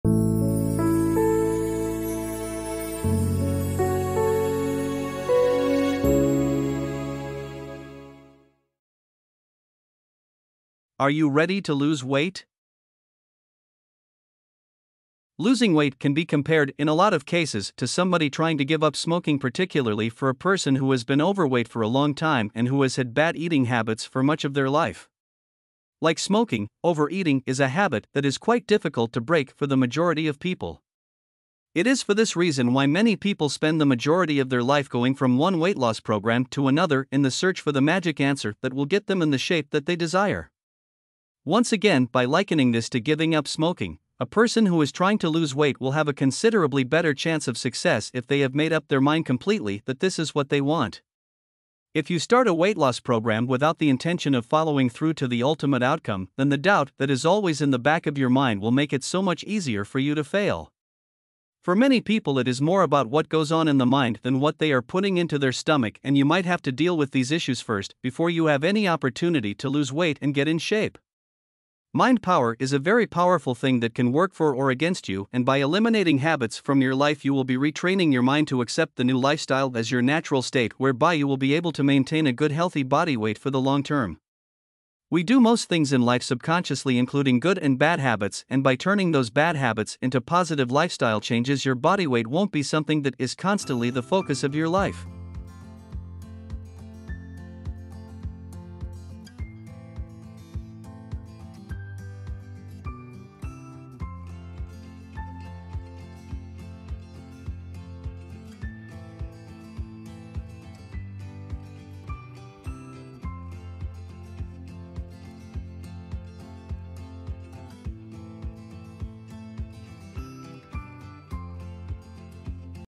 Are You Ready To Lose Weight
Whether you are leading a busy lifestyle or just can't find the time to work out, this video is for you! In this video, I am going to reveal how to get an instant boost of energy and lose weight in just 30 days with these 5 easy tricks.